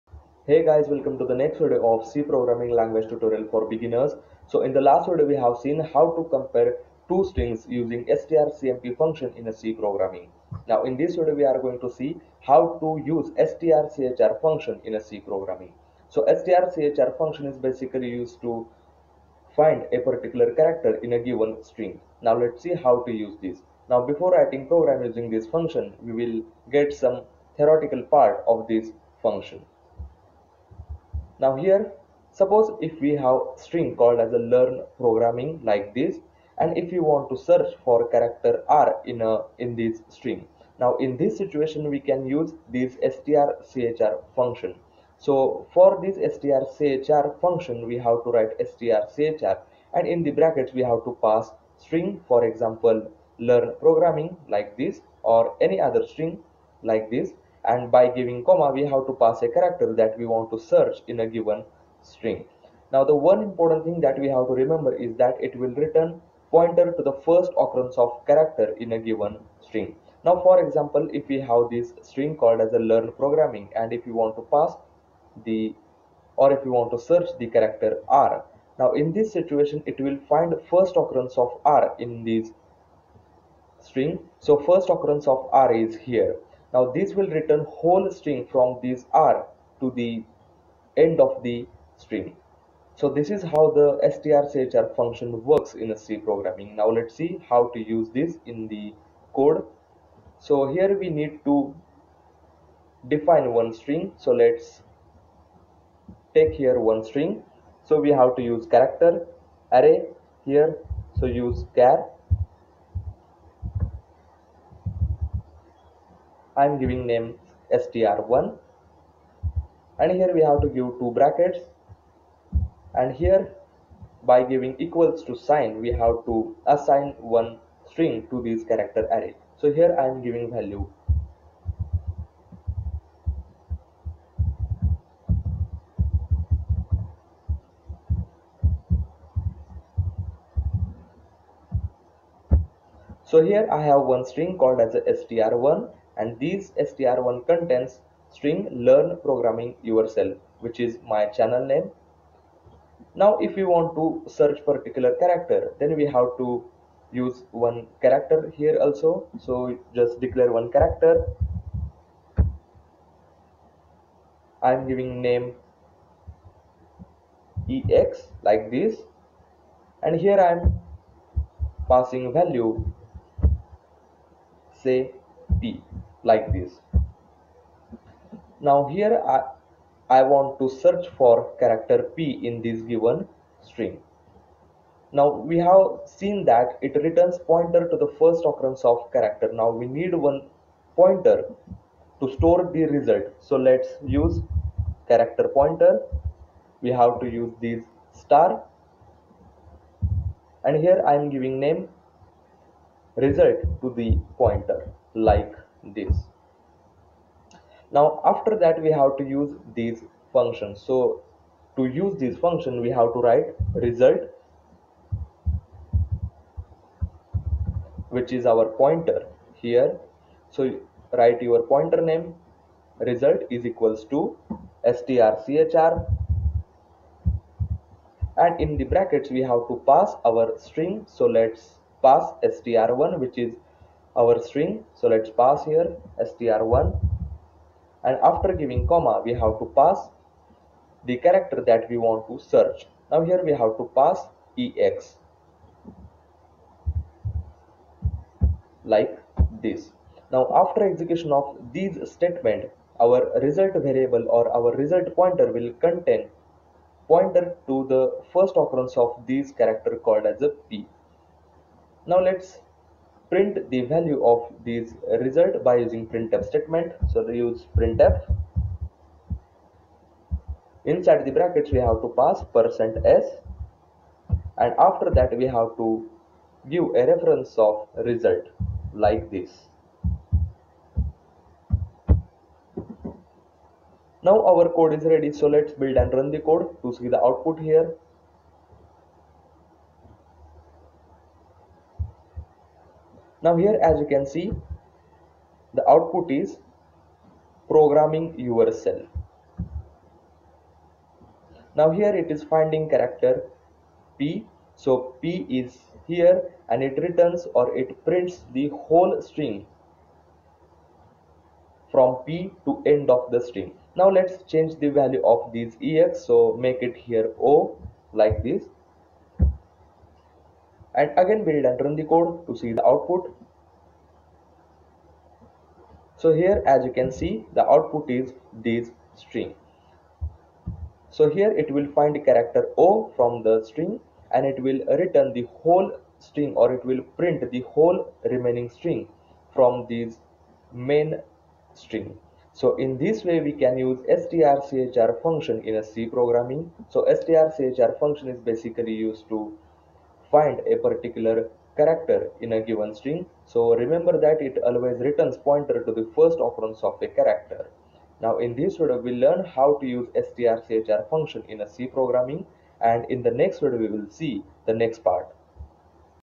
STRCHR() Function in C Programming | Find Character From String Using STRCHR() Function
This is how to use strchr() function to find a character from given string in c programming language.

Links to my other FREE courses.